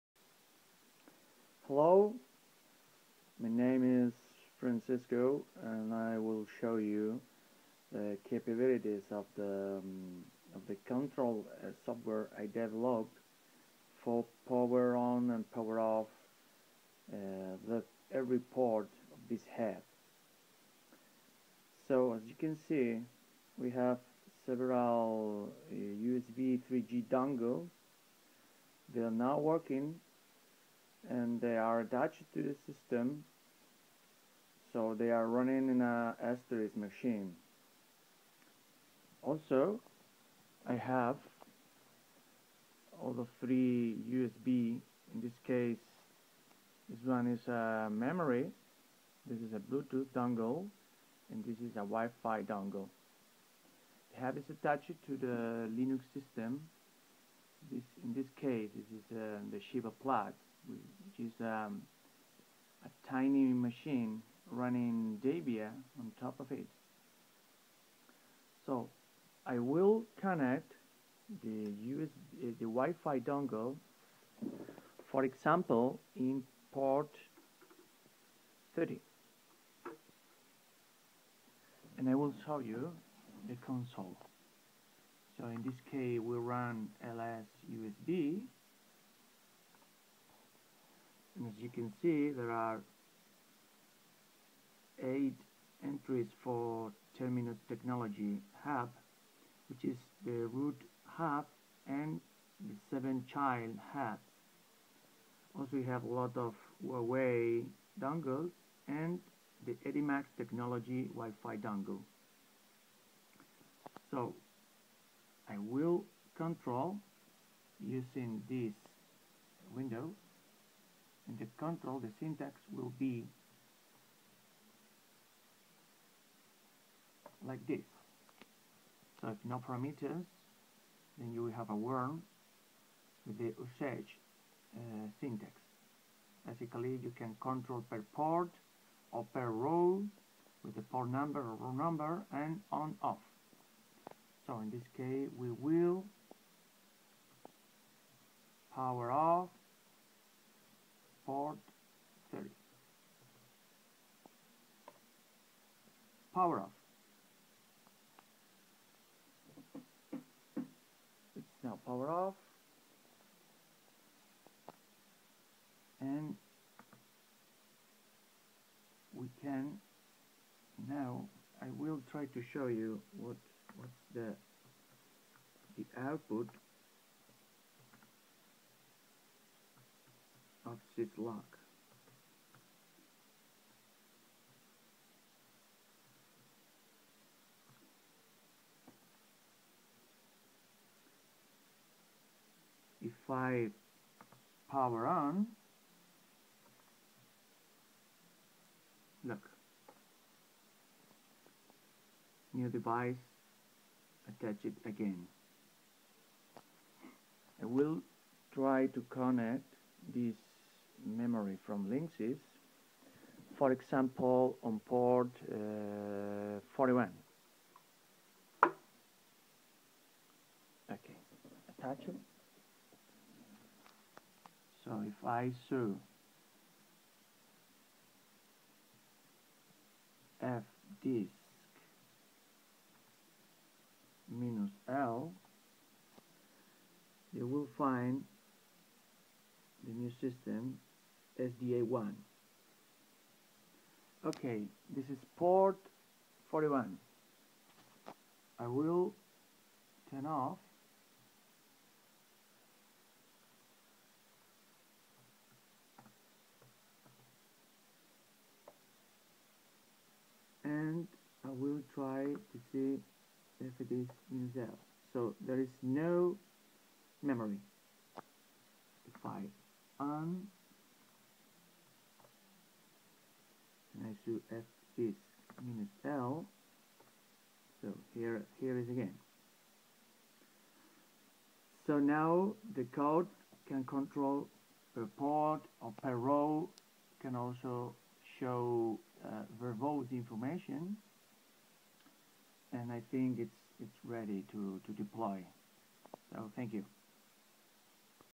Eyeboot 49 Port USB Hub individual Power on/off function using Linux
This video will show you how to remotely turn power on and off for individual USB ports on the Eyeboot 49 Port USB Hub. Using Linux you can power on/off individual USB ports or whole rows of USB ports.